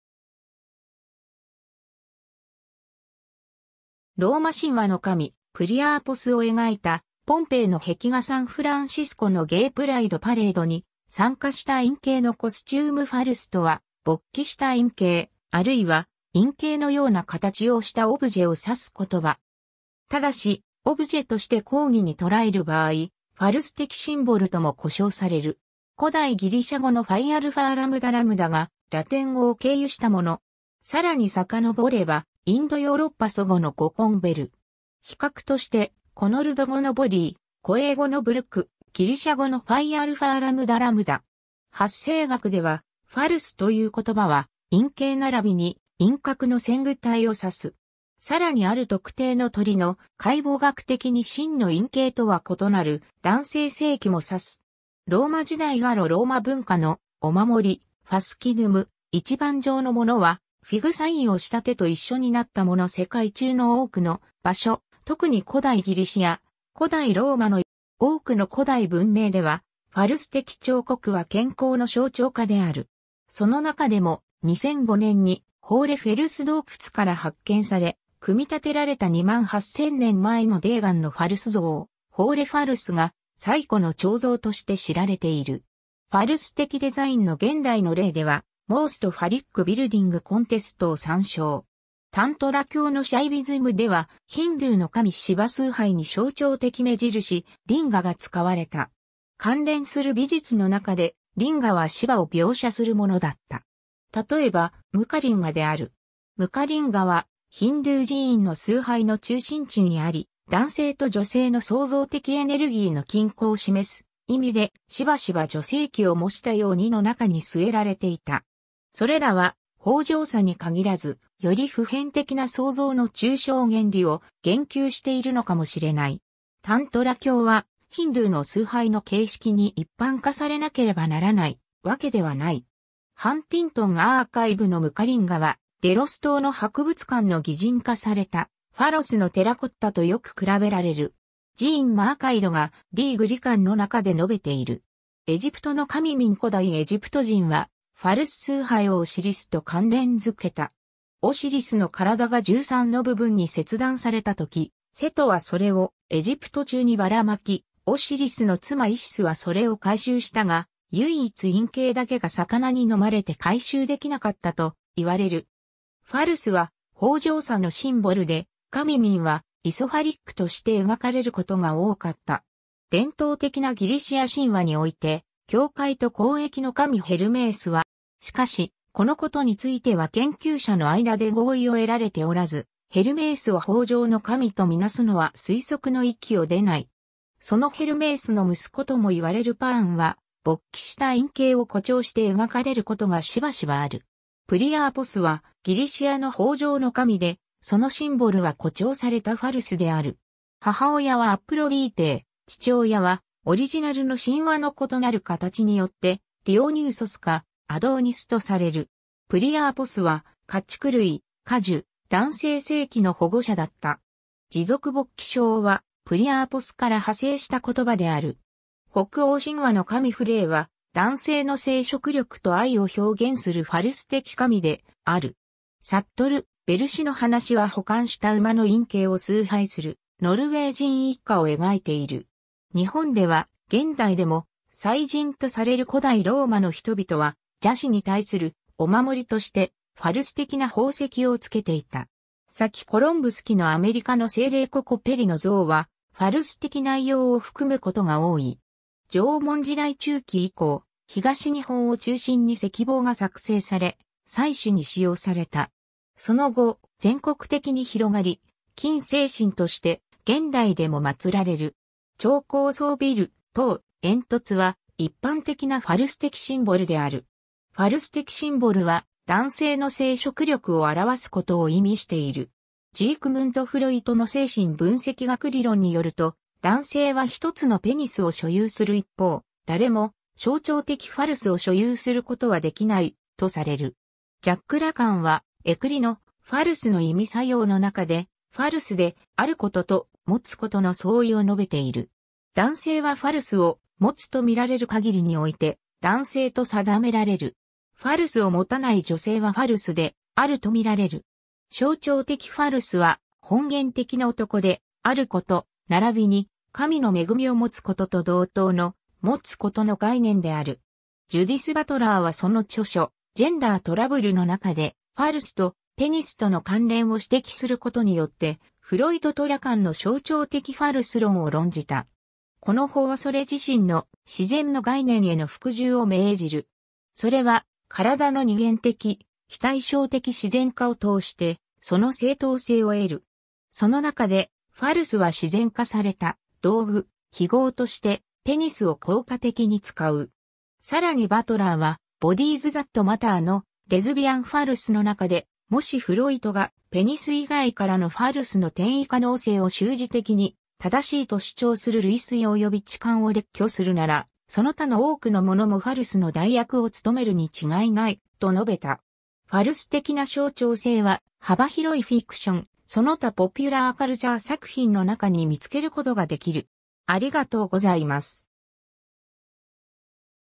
ファルス (性)
ジャック・ラカン
ローマ神話の神プリアーポスを描いたポンペイの壁画（1世紀） サンフランシスコのゲイ・プライド・パレードに参加した陰茎のコスチューム（2005年） ファルス (phallus) とは、勃起した陰茎、あるいは陰茎のような形をしたオブジェを指す言葉。
ただし、オブジェとして広義にとらえる場合、ファルス的シンボル (phallic symbol) とも呼称される。
 古代ギリシャ語のφαλλός（phallós, ファロス、パロス）がラテン語を経由したもの。
さらに遡れば、インド・ヨーロッパ祖語の語根bhel（膨らむこと、膨張）。
比較として、古ノルド語（ならびに現代アイスランド語）のboli（雄牛）、古英語のbulluc（小さな弱い牛）、ギリシャ語のφαλλή（クジラ）。
 発生学では、「ファルス」という言葉は、陰茎ならびに陰核の前駆体を指す。
（Phallus (embryology)参照） さらにある特定の鳥の、解剖学的に真の（たとえば哺乳類の）陰茎とは異なる男性性器も指す。
（Bird anatomy#Reproduction参照） ローマ時代ガロ・ローマ文化のお守りファスキヌム、一番上のものはフィグ・サイン（manus fica）をした手と一緒になったもの 世界中の多くの場所、とくに古代ギリシア・古代ローマの遺物の中で、古代および近代のphalloi（ファルス）の彫刻を見ることができる。
多くの古代文明では、ファルス的彫刻は健康の象徴化である。
その中でも、2005年にホーレ・フェルス洞窟から発見され、組み立てられた2万8千年前の泥岩のファルス像「ホーレ・ファルス」が、最古の彫像として知られている。
 ファルス的デザインの現代の例ではMost Phallic Building contestを参照。
 タントラ教のシャイヴィズムでは、ヒンドゥー (Hindu) の神シヴァ崇拝に象徴的目印「リンガ」が使われた。
関連する美術の中で、リンガはシヴァを描写するものだった。
たとえば、ムカリンガである。
ムカリンガはヒンドゥー寺院 (Hindu temple) の崇拝の中心地にあり、男性と女性の創造的エネルギーの均衡を示す意味で、しばしば女性器を摸したヨーニ (Yoni) の中に据えられていた。
それらは豊饒さに限らず、より普遍的な創造の抽象原理を言及しているのかも知れない。
タントラ教はヒンドゥーの崇拝の形式に一般化されなければならないわけではない。
 Huntington Archiveのムカリンガは、デロス島の博物館の擬人化されたファロスのテラコッタとよく較べられる。
Jean Marcadéが『Die Griechen』の中で述べている。
 エジプトの神ミン 古代エジプト人はファルス崇拝をオシリスと関連づけた。
オシリスの体が13の部分に切断された時、セトはそれをエジプト中にばらまき、オシリスの妻イシスはそれを回収したが、唯一陰茎だけが魚に呑まれて回収できなかったと言われる。
（Legend of Osiris and Isis参照） ファルスは豊饒さのシンボルで、神ミンは「ithyphallic（勃起した陰茎）」として描かれることが多かった。
 伝統的なギリシア神話において、境界と交易の神（一般には伝令の神）ヘルメースは、ファルスを摸したヘルマと呼ばれる柱との関係から、ファルス的な神と見なされている。
しかし、このことについては研究者の間で合意を得られておらず、ヘルメースを豊饒の神と見なすのは推測の域を出ない。
 そのヘルメースの息子とも言われるパーンは、勃起した陰茎を誇張して描かれることがしばしばある。
 プリアーポス (Priapus) はギリシアの豊饒の神で、そのシンボルは誇張されたファルスである。
母親はアプロディーテー、父親はオリジナルの神話の異なる形によってディオニューソスかアドーニスとされる。
プリアーポスは家畜類・果樹・男性性器の保護者だった。
「持続勃起症 (Priapism) 」は「プリアーポス」から派生した言葉である。
 北欧神話の神フレイは、男性の生殖力と愛を表現するファルス的神である。
 サットル『ヴェルシの話』は保管した馬の陰茎を崇拝するノルウェー人一家を描いている。
 日本では、現代でも祭神とされる（神戸市西区・裸石神社） 古代ローマの人々は、邪視に対するお守りとしてファルス的な宝石を着けていた。
 先コロンブス...